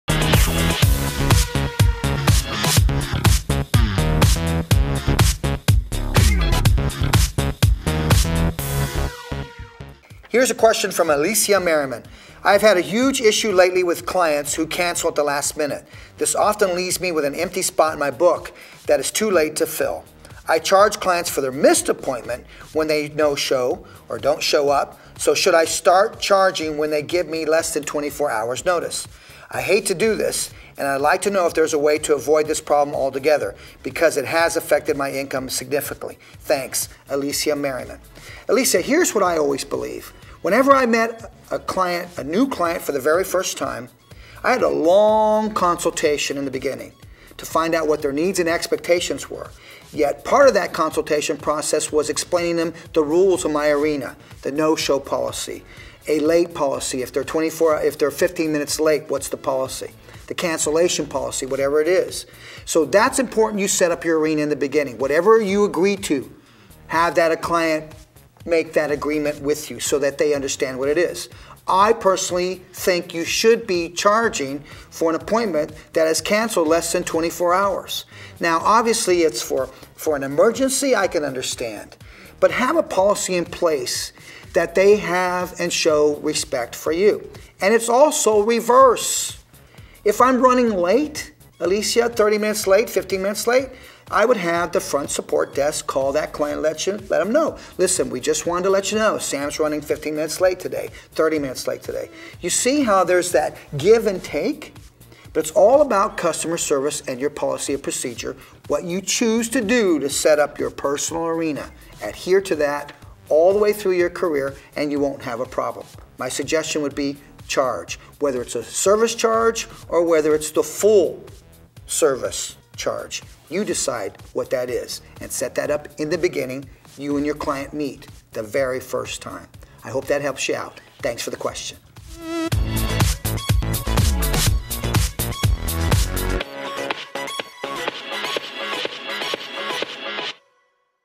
How To Handle Last Minute Salon Cancellations
As a professional hairdresser, should you charge for cancellations that are done with less than 24 hours notice? How do you establish a policy with your clientele? Sam Villa talks about setting yourself up for success in this latest Q&A video!

Everything You Need To Know About Proper Salon Etiquette